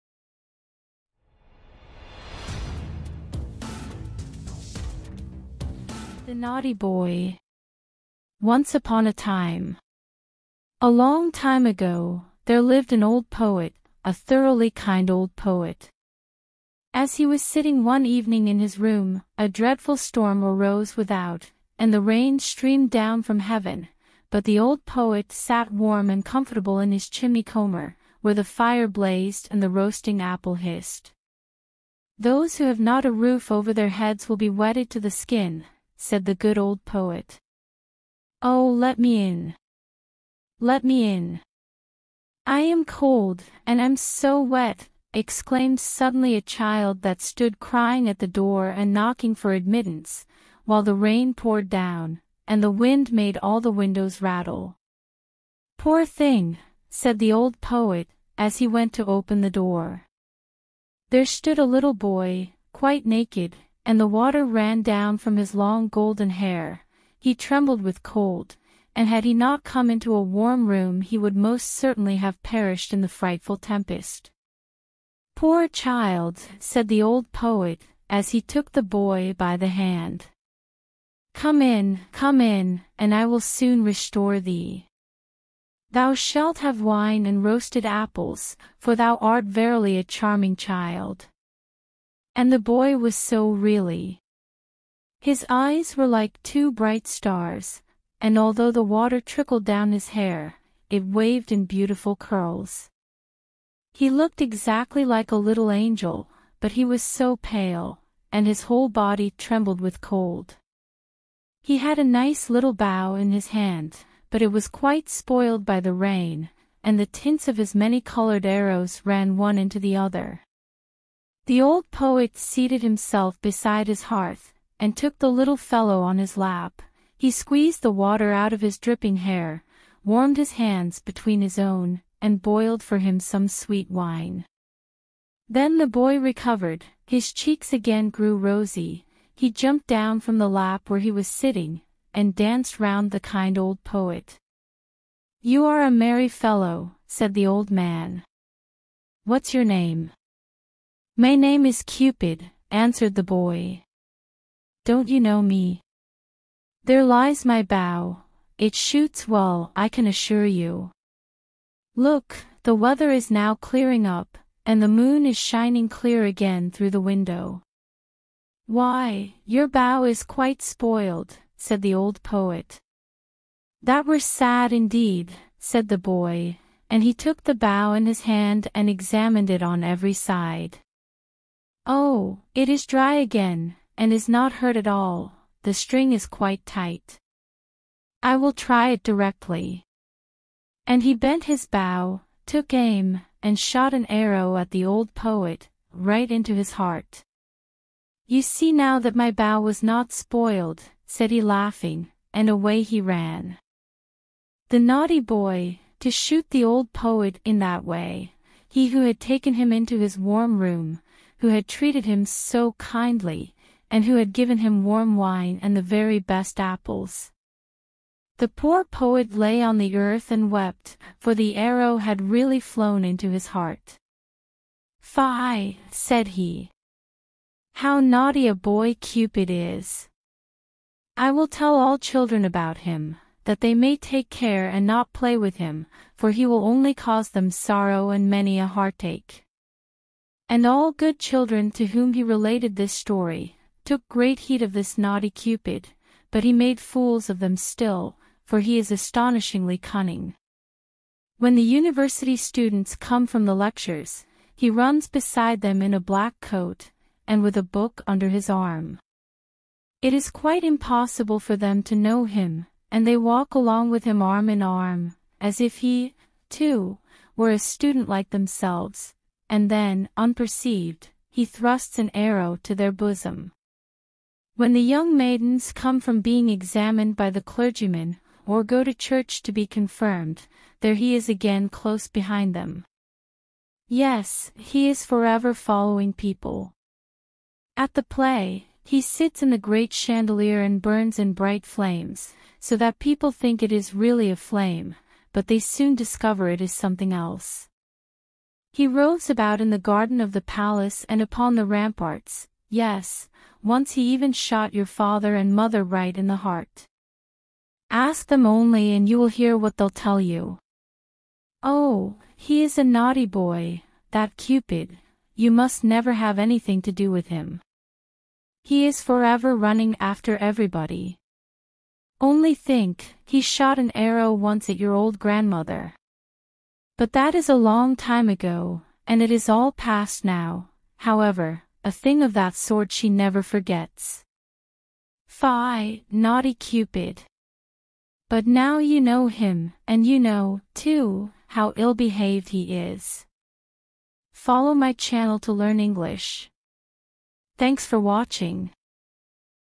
Improve your English through story | Practice English | English Speaking Course | Learn English
Hello guys !
In this video, a new story entitled "The Naughty Boy" is presented. the story is so interesting and so easy to understand. So it is quite useful to learn English and its comprehension, at the same time it will help you to rectify your pronunciation and beautify your English speaking. Do watch the video till the end. Thanks